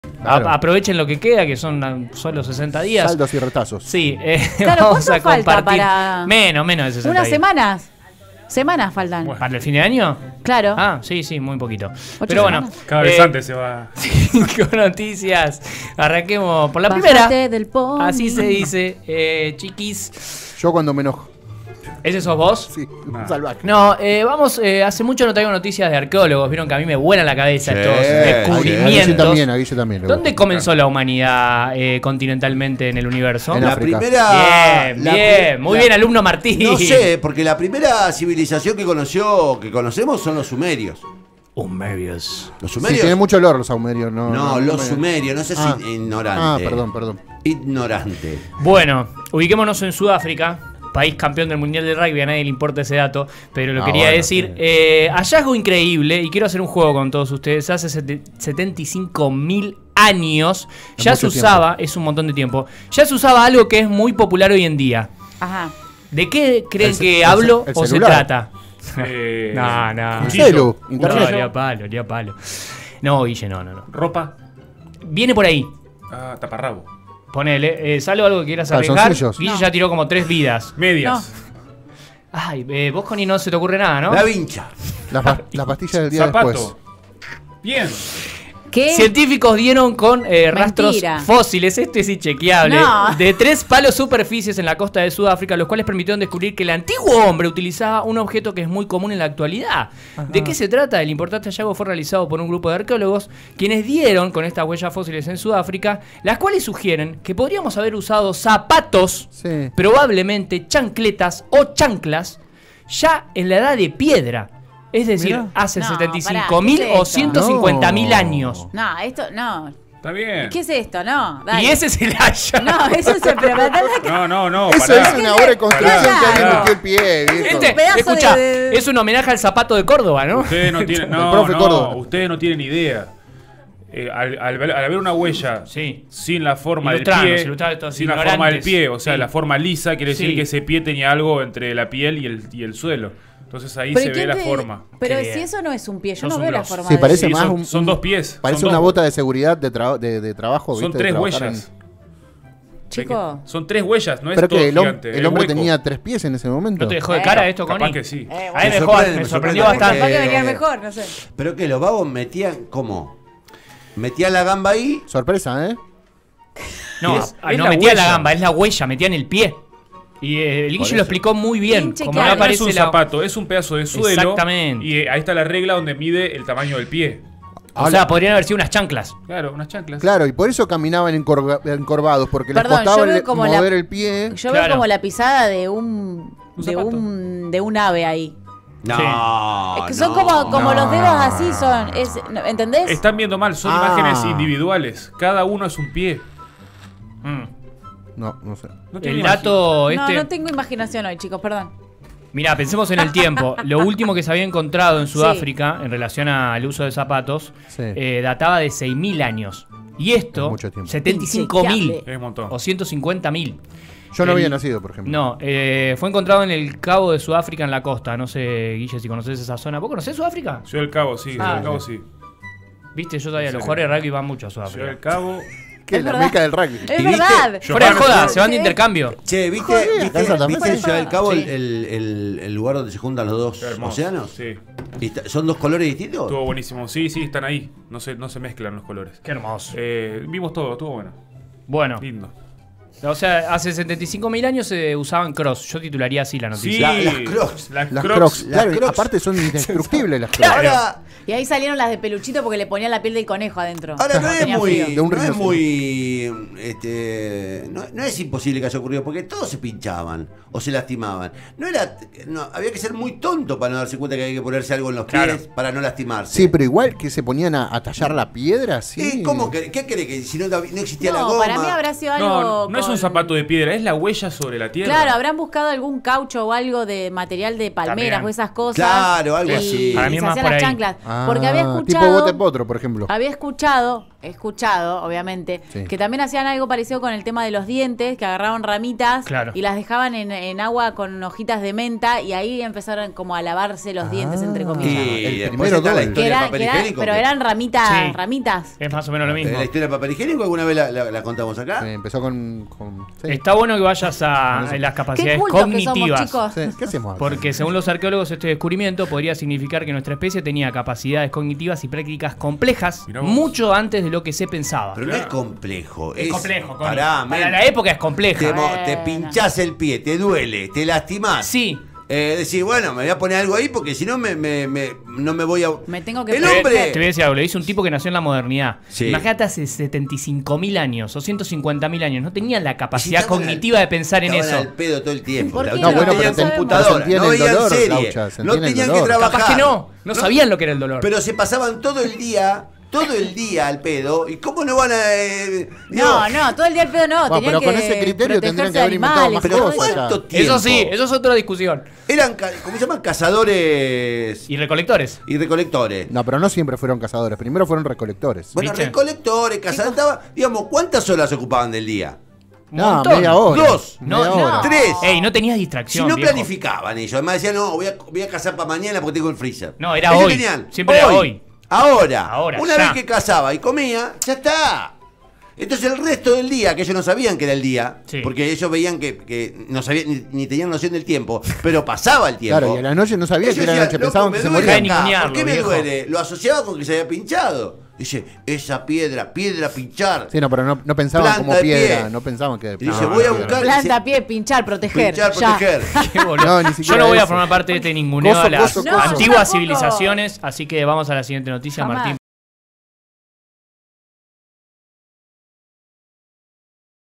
Hallazgo increíble: hace 75.000 años ya se usaba algo que es muy popular hoy en día
Científicos dieron con rastros de fósiles en tres paleosuperficies en la costa de Sudáfrica, los cuales permitieron descubrir que el "antiguo hombre" utilizaba un objeto que es muy común en la actualidad.